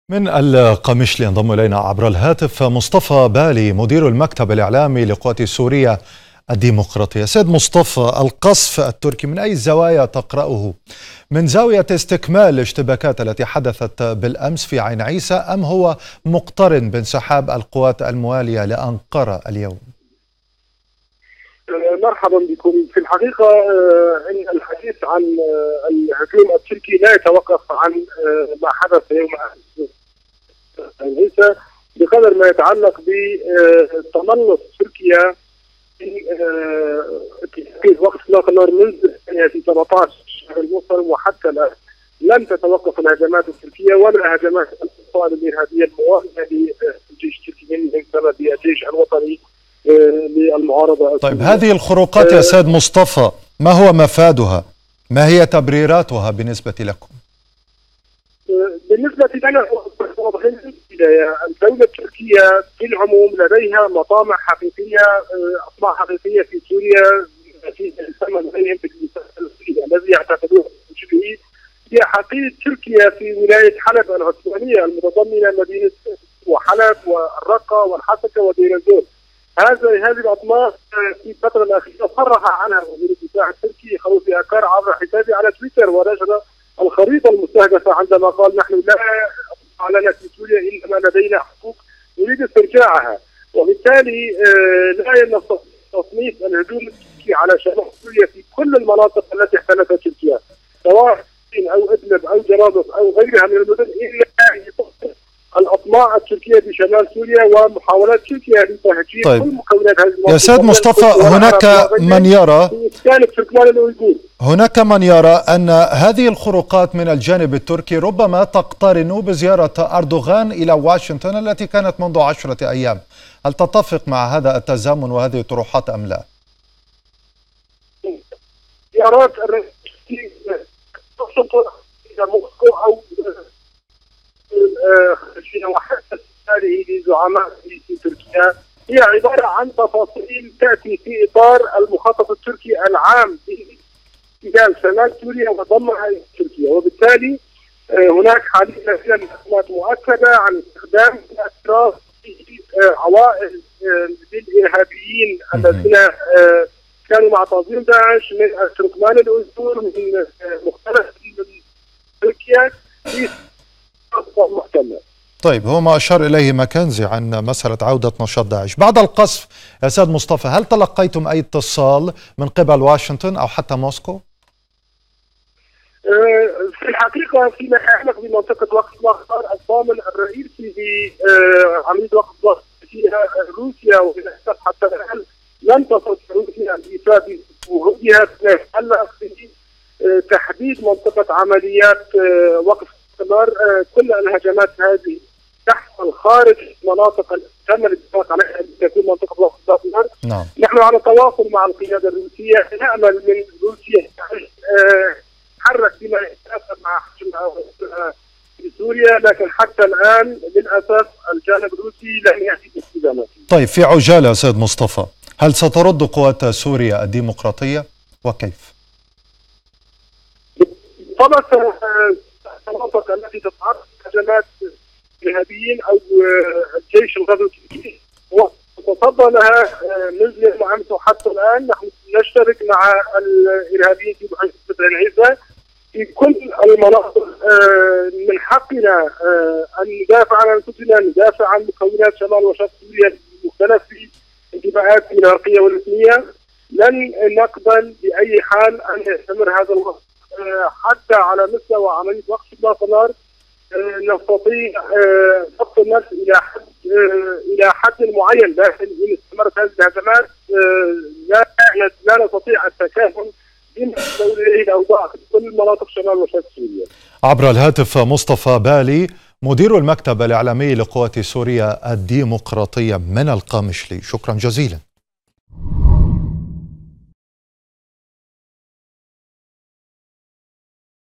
شاهد.. أول تعليق من قوات سوريا الديمقراطية على القصف التركي لمخيم عين عيسى
أفاد المرصد السوري باستهداف طائرة مسيرة تركية لـ مخيم_عين_عيسى شمال الرقة بعد انسحاب المسلحين الموالين لـ أنقرة من كل المواقع التي سيطرت عليها على مشارف عين عيسى.. مزيد من التفاصيل يتحدث من القامشلي مصطفى بالي مدير المكتب الإعلامي لـ قوات_سوريا_الديمقراطية مع الإعلامي محمد عبد الله.